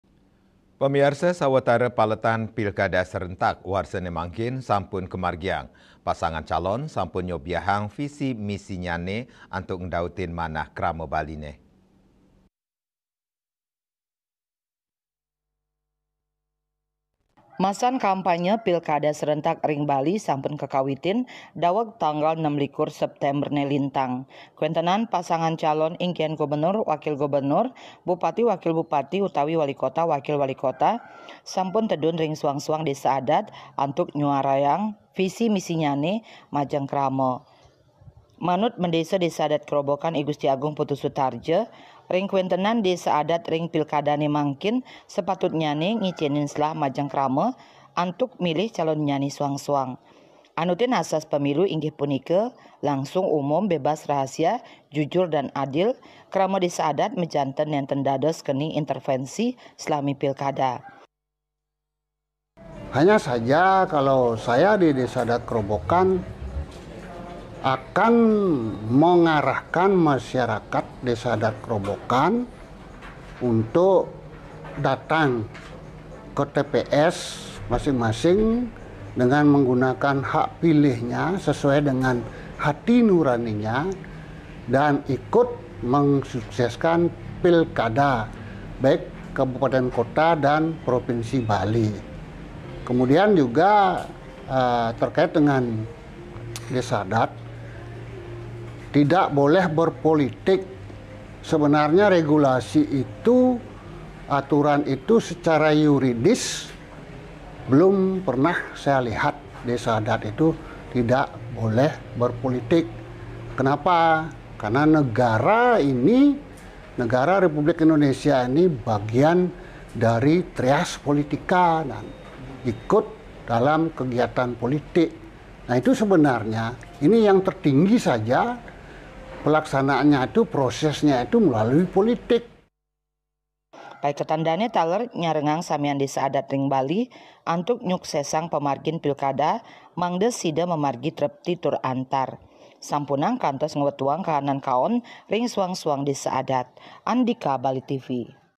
NYELEHIN KAWENTENAN DESA ADAT RING PILKADA 2024
SAWATARA PALETAN PILKADA SERENTAK WARSANE MANGKIN SAMPUN KAMARGIANG,,PASANGAN CALON SAMPUN NYOBIAHANG VISI MISINYANE,ANTUK NGDAUTIN MANAH KRAMA BALINE,,